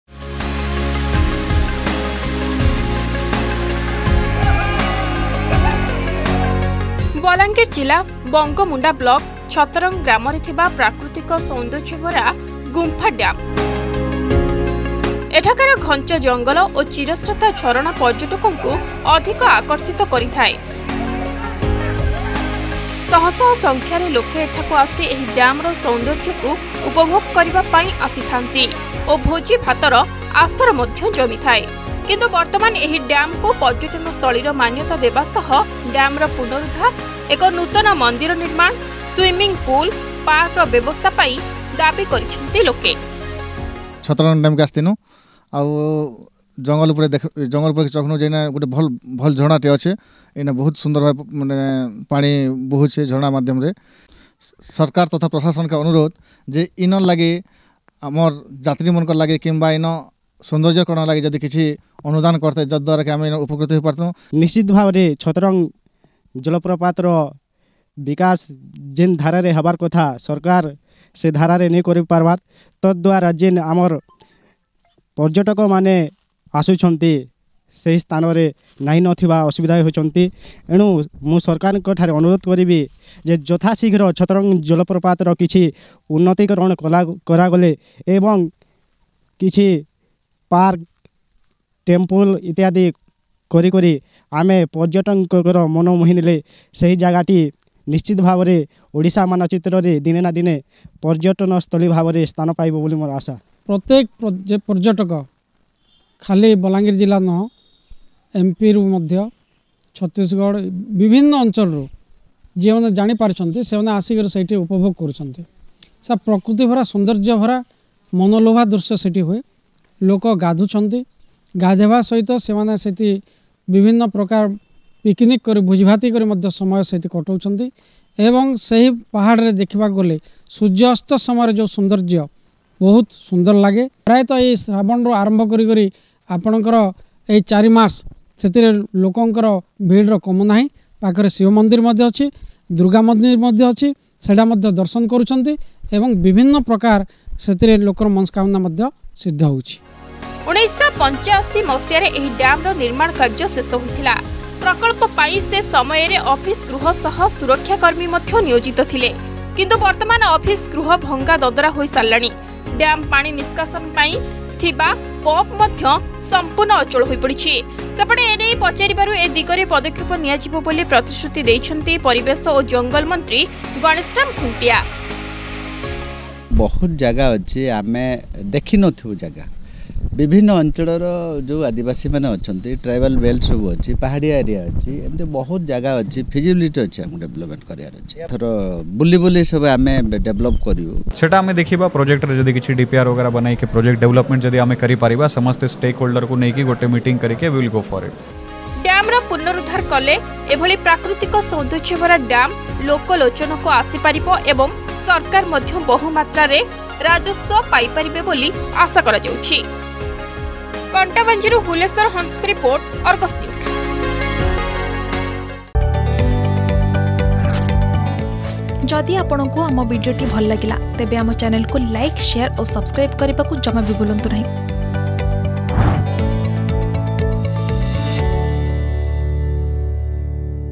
ବଲାଙ୍ଗିର ଜିଲ୍ଲା ଛତରଙ୍ଗ ଗ୍ରାମରେ ଥିବା ପ୍ରାକୃତିକ ସୌନ୍ଦର୍ଯ୍ୟ ଭରା ଗୁମ୍ଫା ଡ୍ୟାମ୍ ।
ବଲାଙ୍ଗିର ଜିଲ୍ଲା ବଙ୍ଗୋମୁଣ୍ଡା ବ୍ଲକ ଛତରଙ୍ଗ ଗ୍ରାମରେ ଥିବା ପ୍ରାକୃତିକ ସୌନ୍ଦର୍ଯ୍ୟ ଭରା ଗୁମ୍ଫା ଡ୍ୟାମ୍ । ଏଠାକାର ଘଞ୍ଚ ଜଙ୍ଗଲ ଓ ଚିରସ୍ରୋତା ଝରଣା ପର୍ଯ୍ୟଟକଙ୍କୁ ଅଧିକ ଆକର୍ଷିତ କରିଥାଏ । କିନ୍ତୁ ବର୍ତ୍ତମାନ ଏହି ଡ୍ୟାମକୁ ପର୍ଯ୍ୟଟନସ୍ଥଳୀର ମାନ୍ୟତା  ଦେବା ସହ ଡ୍ୟାମର ପୁନରୁଦ୍ଧାର, ପାଇଁ ଦାବି କରିଛନ୍ତି ଲୋକେ ।